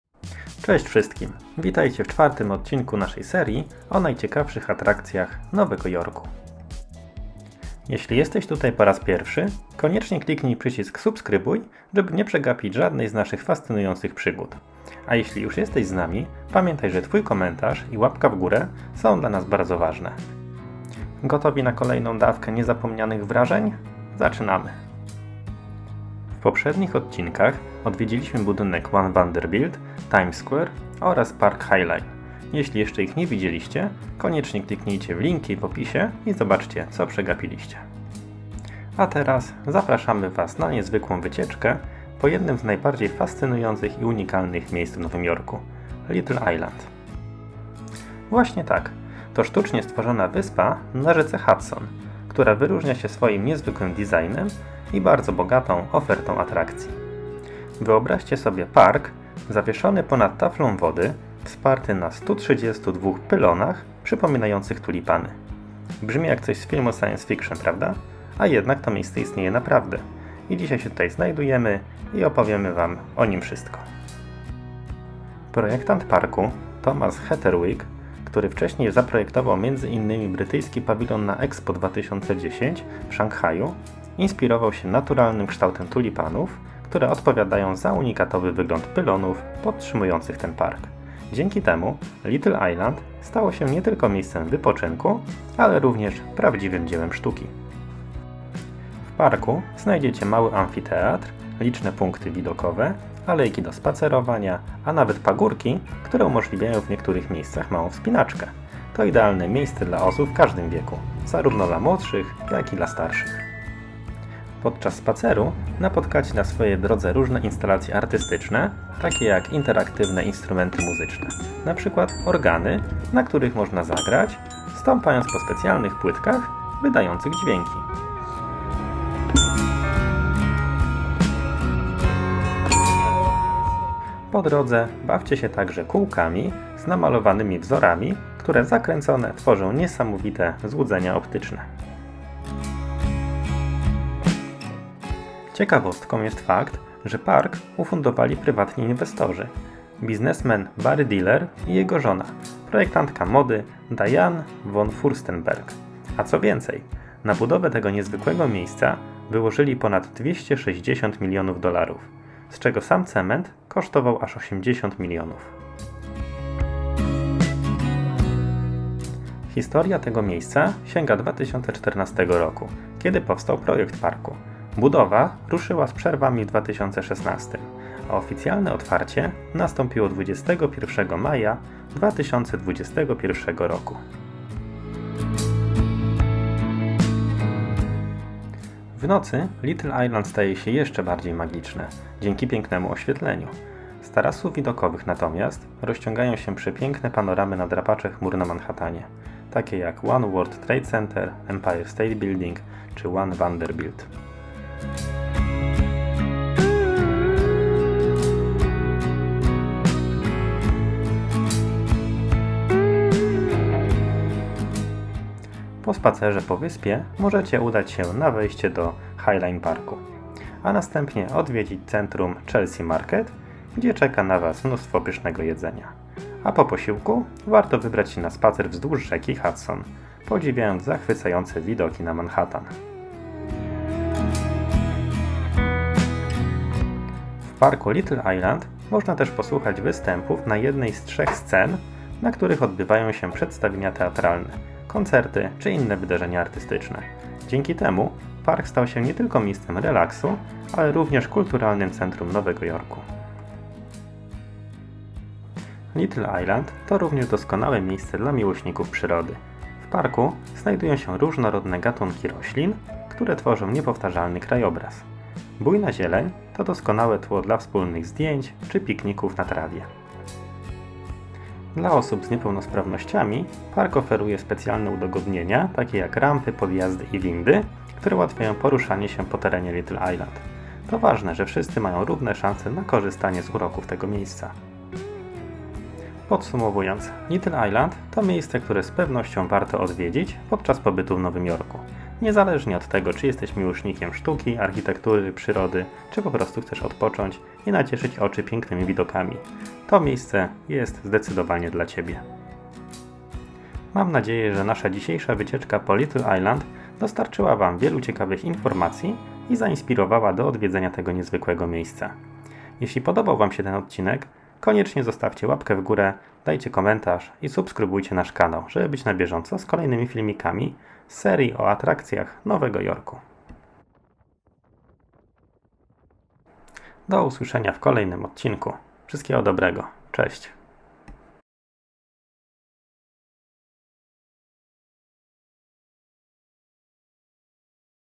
Atrakcje Nowego Jorku - odc. 4 - Little Island Park - Park na wodzie!
Cześć! 🤩 W dzisiejszym odcinku naszej serii o najciekawszych atrakcjach Nowego Jorku, zabierzemy Was na niezwykłą wycieczkę po Little Island - sztucznej wyspie na rzece Hudson, której nie można przegapić! Poznajcie wszystkie sekrety tego magicznego miejsca, gdzie znajdziecie park zawieszony na pylonach przypominających tulipany, fascynujące instalacje artystyczne oraz przepiękne panoramy na drapacze chmur Manhattanu. 🌇🌳🌷

Dziękujemy za oglądanie i do zobaczenia w kolejnym odcinku! 🎉